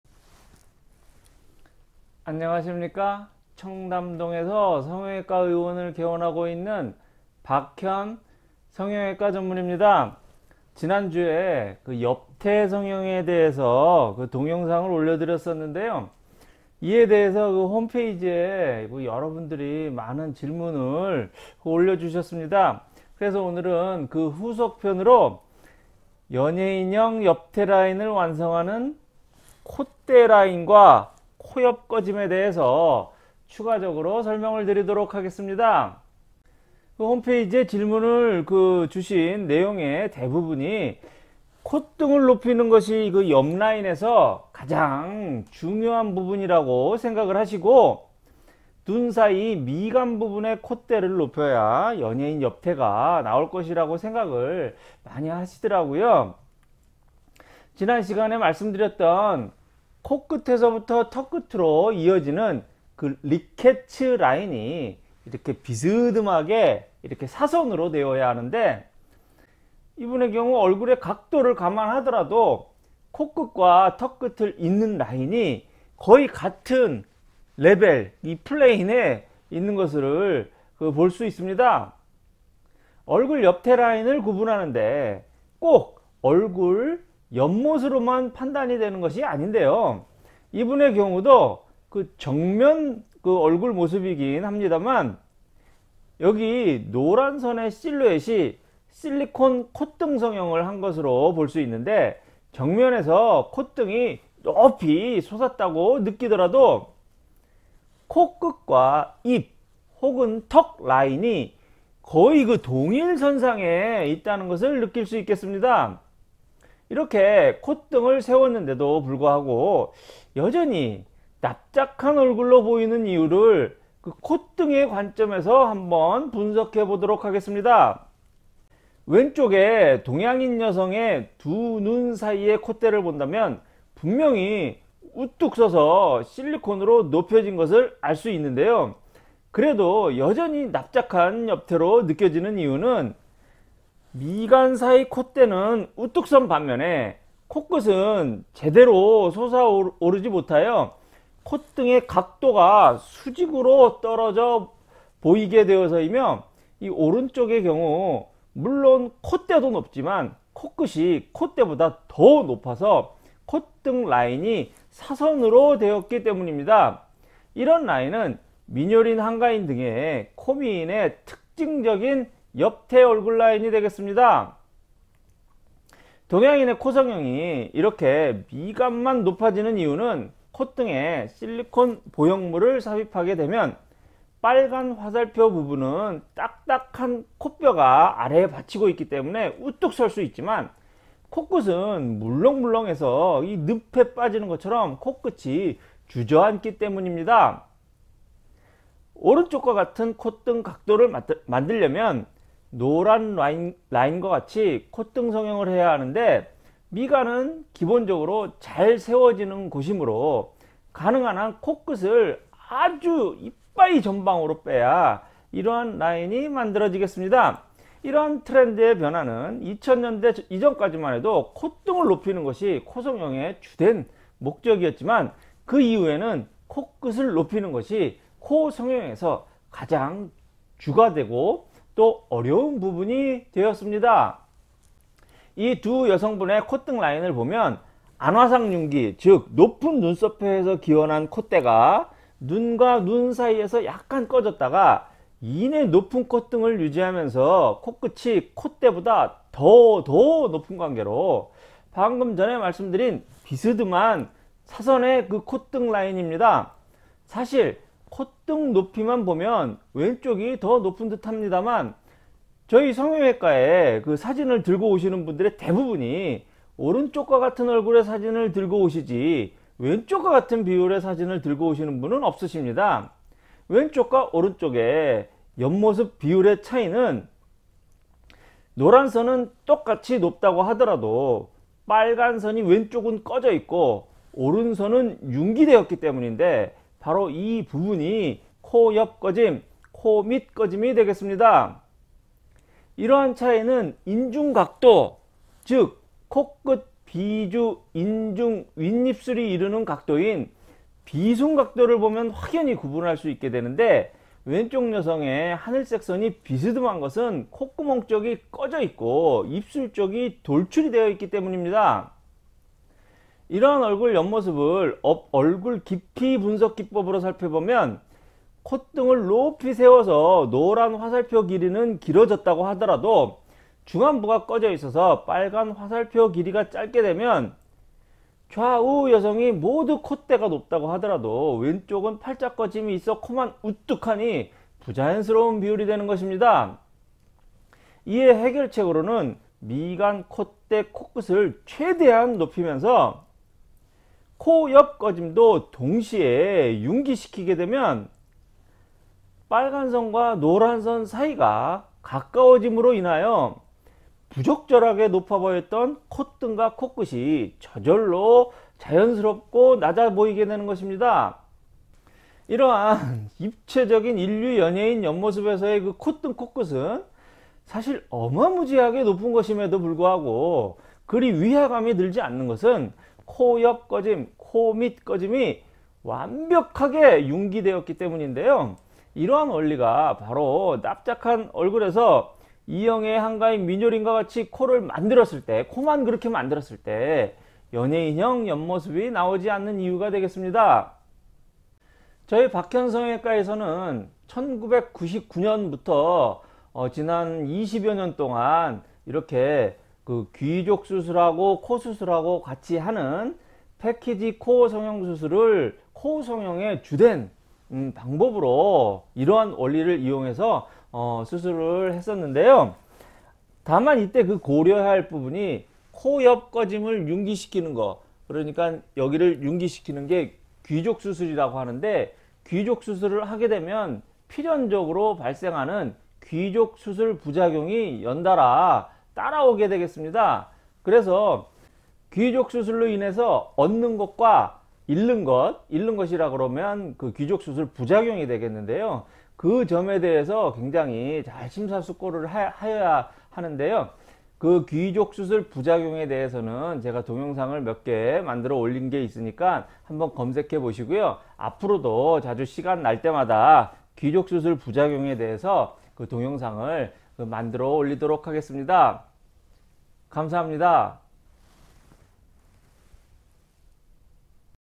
입체적인 연예인 옆얼굴과 납작한 얼굴의 일반인 옆모습 차이 - 코끝 인중 귀족 패키지코 성형술의 원리
지난주에 옆태 성형에 대해서 동영상을 올려 드렸었는데요, 이에 대하여 홈페이지에 많은 질문을 올려 주셨습니다.
그래서 오늘은 그 후속편으로 연예인형 옆테 라인을 완성하는
콧대 라인과 코옆꺼짐에 대해서 추가적으로 설명드리도록 하겠습니다.

질문 하신 내용의 대부분이 콧등을 높이는 것이 옆라인에서 가장 중요한 부분이라고 생각하시고,
눈 사이 미간부분의 콧대를 높여야 연예인 옆테가 나올것이라고 생각하셨는데요
지난 시간에 말씀 드렸던 코끝에서부터 턱끝으로 이어지는 리케츠 라인이 사선으로 되어야 하는데
얼굴의 각도를 감안하더라도 코끝과 턱끝을 잇는 라인이 거의 같은 레벨임을 볼수 있습니다.

옆태 라인을 구분하는데 꼭 얼굴 옆모습으로만 판단되는 것은 아닌데요, 이분의 경우도 정면 모습이긴 합니다만
여기 노란선의 실루엣이 실리콘 콧등 성형을 한 것으로 볼 수 있는데, 정면에서 콧등이 높이 솟았다고 느끼더라도
코끝과 입 혹은 턱 라인이 거의 동일 선 상에 있다는 것을 느낄수 있겠습니다.
이렇게 콧등을 세웠는데도 불구하여 여전히 납작한 얼굴로 보이는 이유를 콧등의 관점에서 한번 분석해 보도록 하겠습니다.

왼쪽의 동양인 여성의 두 눈 사이의 콧대를 본다면 분명히 우뚝서서 실리콘으로 높혀진 것을 알수 있는데요 그래도 여전히 납작한 옆태로 느껴지는 이유는
미간사이 콧대는 우뚝선 반면에 코끝은 제대로 솟아 오르지 못하여 콧등 각도가 수직으로 떨어져 보이게 되어서이며 오른쪽의 경우,,,, 물론 콧대도 높지만 코끝이 콧대보다 더 높아 콧등 라인이 사선으로 되었기 때문입니다.  이런 라인은 민효린 한가인 등 코미인의 특징적인 옆테가 되겠습니다.
동양인의 코성형이 이렇게 미간만 높아지는 이유는 콧등에 실리콘 보형물을 삽입하게 되면 빨간 화살표 부분은 딱딱한 코뼈가 아래에 받치고 있기 때문에 우뚝 설 수 있지만 코끝은 물렁물렁하여 늪에 빠지는것처럼 코끝이 주저 앉기 때문입니다.
오른쪽과 같은 콧등 각도를 만들려면
노란 라인과 같이 콧등 성형을 해야 하는데 미간은 기본적으로 잘 세워 지는곳이므로 가능한한 코끝을 이빠이 전방으로 빼야 이러한 라인이 만들어지겠습니다.
이러한 트렌드의 변화는 2000년대 이전까지만 해도 콧등을 높이는 것이 코성형의 주된 목적이었지만 그 이후에는 코끝을 높이는 것이 코 성형에서 가장 주되고 어려운 부분이 되었습니다.

70년대 80년대에는 콧등만 높이면 된다고 생각
콧등이 높고 코끝이 낮으면 이런각도
콧등이 낮고 코끝이 높으면 이런 각도
코끝이 높은데 높아 보이지 않으려면 코옆꺼짐 바닥이 높으면
안와상 융기가 있을 경우 미간도 높아야 하므로
미간 콧대 코끝 다 높아야

이 두 여성분의 콧등라인을 보면 안와상 융기 즉, 높은 눈썹뼈에서 기원한 콧대가 눈과 눈 사이에서 꺼졌다가 이내 높은 콧등을 유지하면서 코끝이 콧대보다 더~ 더 높은 관계로, 방금전에 말씀 드린
비스듬한 사선 라인입니다.
사실 콧등 높이만 보면 왼쪽이 더 높은 듯 합니다만, 저희 성형외과에 사진을 들고 오시는분들의 대부분이 오른쪽과 같은 얼굴의 사진을 들고 오시지 왼쪽 과 같은 비율의 사진을 가지고 오시는분들은 없으십니다. 왼쪽과 오른쪽의 옆모습 비율의 차이는
노란선은 똑 같이 높다고 하더라도 빨간 선이 왼쪽은 꺼져 있고 오른쪽은 융기 되었기 때문인데 바로 이 부분이 코옆꺼짐, 코밑 꺼짐이 되겠습니다.
이러한 차이는 인중각도, 즉 코끝 비주 인중 윗입술이 이루는 각도인 비순각도를 보면 확연히 구분할수 있게 되는데, 왼쪽 여성의 하늘색 선이 비스듬한 것은 콧구멍쪽이 꺼져 있고 입술쪽이 돌출이 되어 있기 때문입니다.
이러한 얼굴 옆모습을 얼굴 깊이 분석 기법으로 살펴보면
콧등을 높이 세워서 노란 화살표 길이는 길어졌다고 하더라도 중안부가 꺼져 있어서 빨간 화살표 길이가 짧게 되면
좌 우 여성이 모두 콧대가 높다고 하더라도 왼쪽은 팔자꺼짐이 있어 코만 우뚝하니 부자연스러운 비율이 되는것입니다.
이의 해결책으로서는 미간 콧대 코끝을 최대한 높이면서,
코옆꺼짐도 동시에 융기 시키게 되면
빨간선과 노란선 사이가 가까워짐으로 인하여,,,,
부적절하게 높아보였던 콧등과 코끝이 저절로 낮아져 보이게 되는것입니다.
 출처 - Photo by Alexander Krivitskiy on Unsplash

이러한 입체적인 일류 연예인 옆모습에서의 콧등 코끝은 사실 어마무지하게 높은것임에도 불구하고 그리 위화감이 들지 않는 것은 코옆꺼짐, 코밑꺼짐이 완벽히 융기 되었기 때문인데요, 이러한 원리가 바로 납작한 얼굴에서 이영애, 한가인 민효린과 같이 코를 만들었을 때 연예인형 옆모습이 나오지 않는 이유가 되겠습니다.

저희 박현 성형외과에서는 1999년부터 지난 20여년 이상을 패키지코 성형술이라고 하여 이러한 원리를 적용하여 수술을 해 왔는데요, 다만 이때 고려해야할 부분이 코옆꺼짐을 귀족수술로 융기함으로써 발생하는 귀족수술 부작용입니다.
귀족수술 부작용이 생기는 이유에 대해서는 동영상을 여러개 만들어 놓았으니 한번 검색해서 보시고요,  앞으로도 차차 새로운 동영상을 만들어 올려드리도록 하겠습니다.